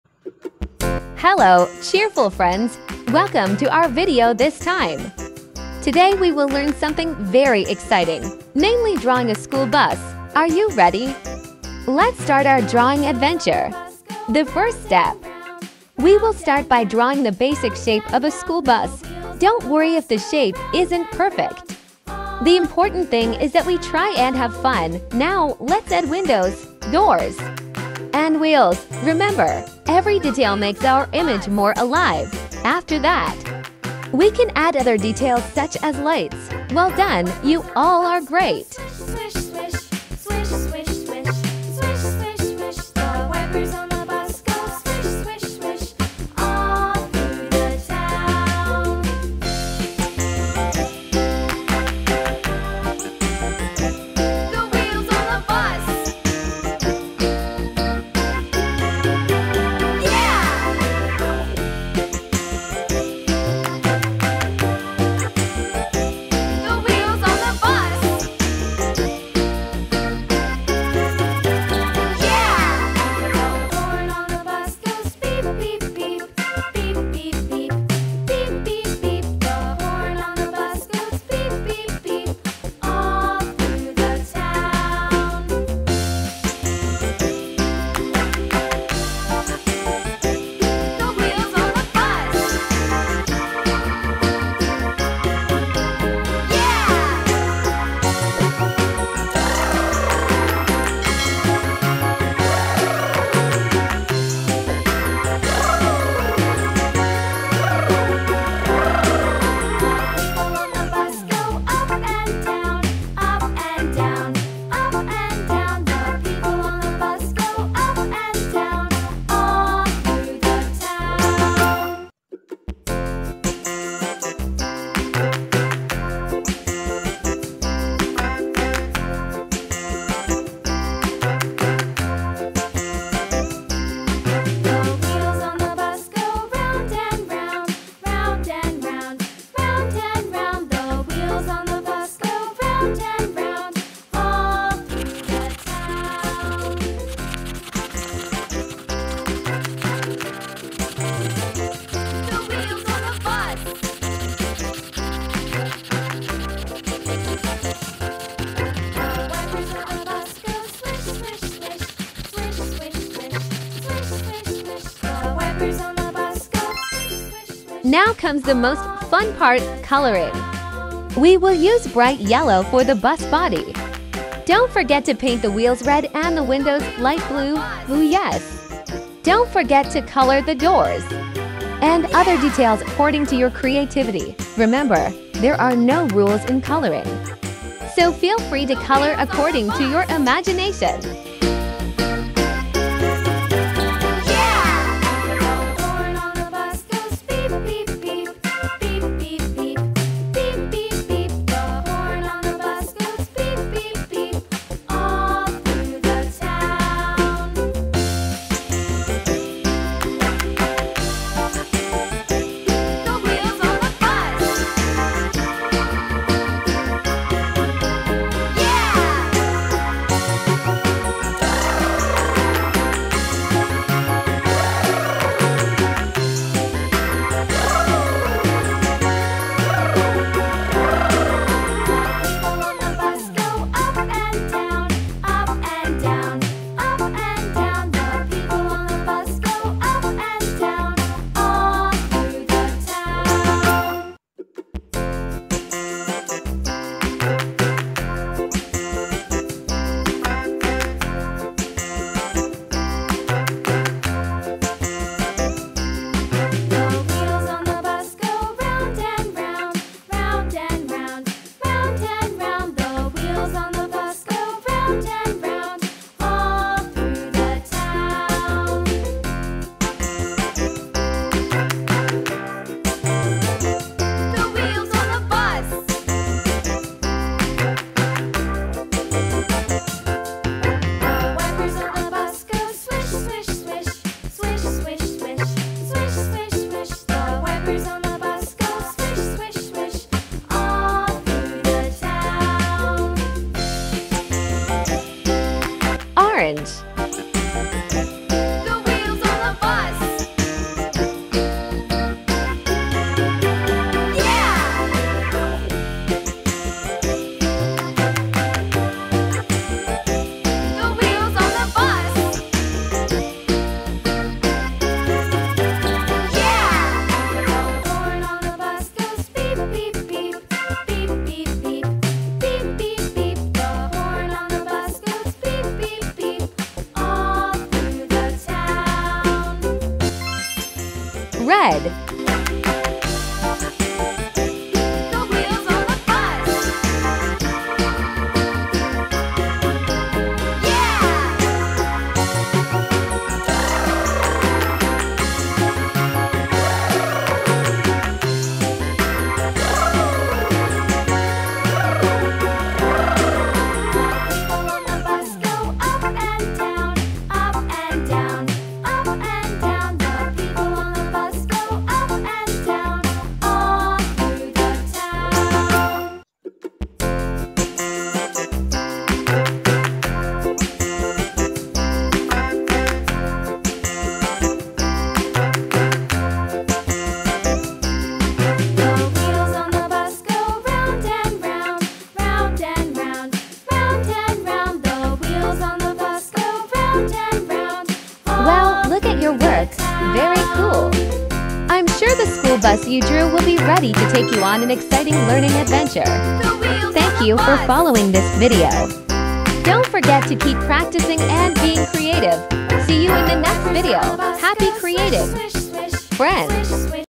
Learn to Draw a School Bus, Fun and Exciting | For children, toddlers, coloring, section 105
Welcome to our learn to draw video! This time, we will take our imagination and creativity to the next level by learning to draw a school bus. Not only that, we will also color our pictures with cheerful colors. This video is designed for children to learn while playing and being creative.

In this video, we will go through each step in detail, from drawing basic shapes, adding details, to coloring. So, don't worry if you are just learning to draw. Remember, the most important thing is the learning process and having fun!

So, prepare your painting tools and favorite colors, and let's start this drawing adventure! Happy creativity!